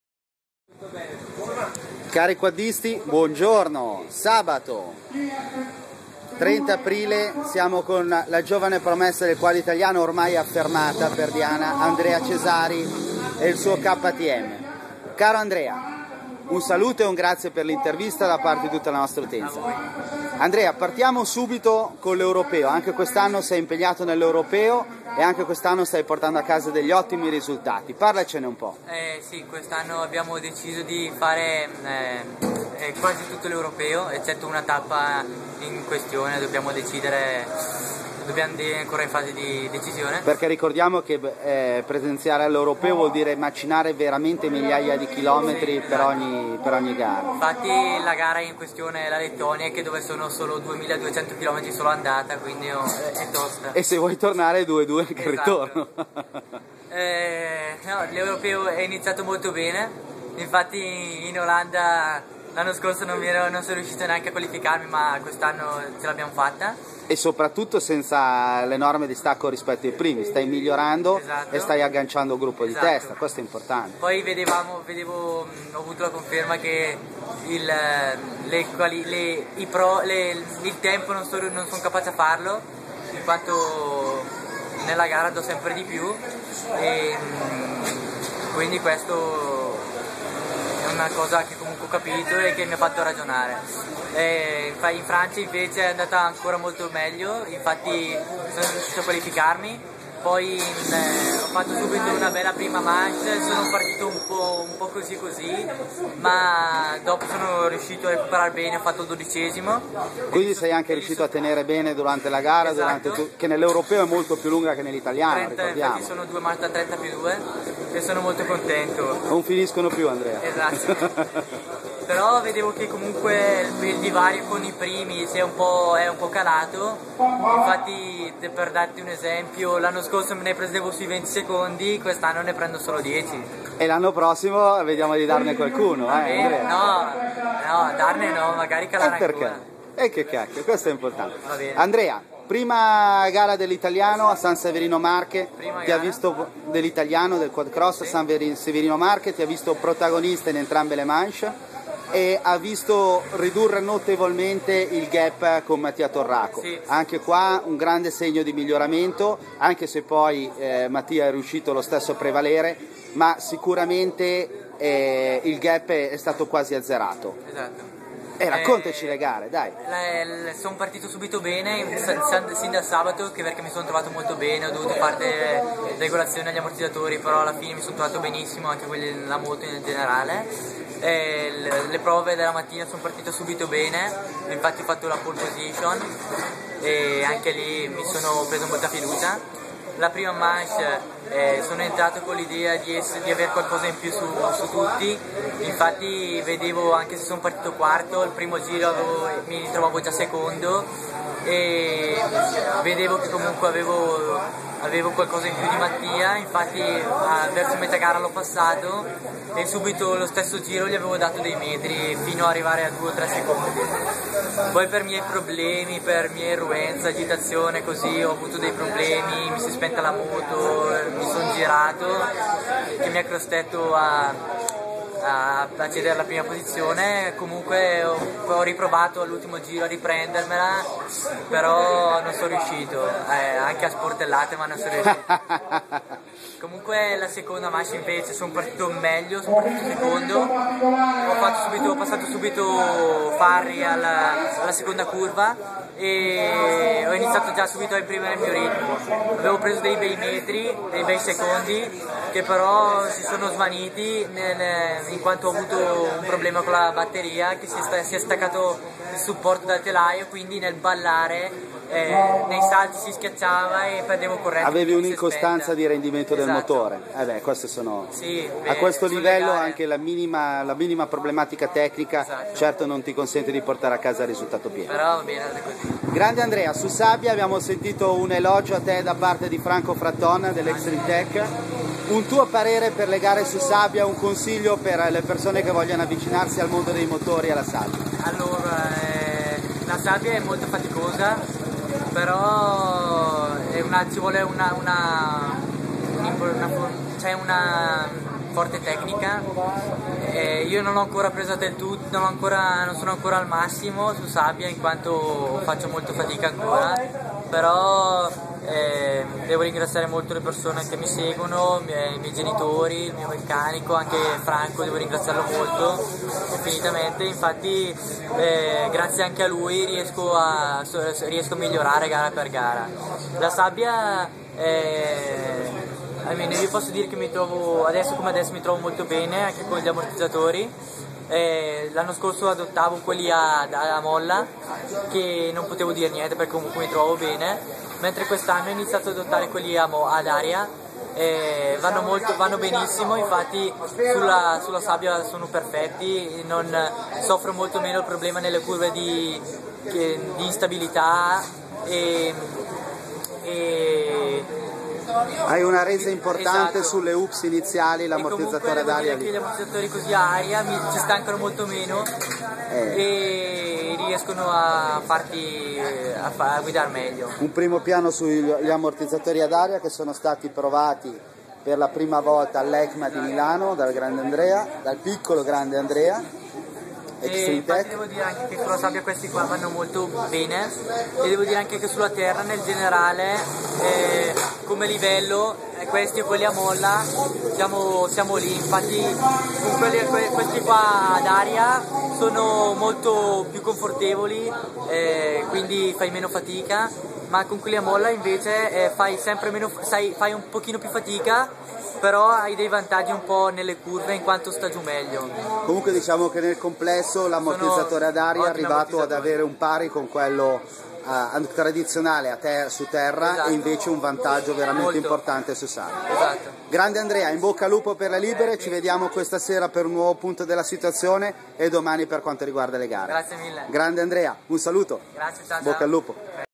2011-04-30__cesari_sabato_fano_quadcross.3gp
Super quad show 2011, intervista a Andrea Cesari prove del sabato Quad Cross, diritti di Quaddisti.it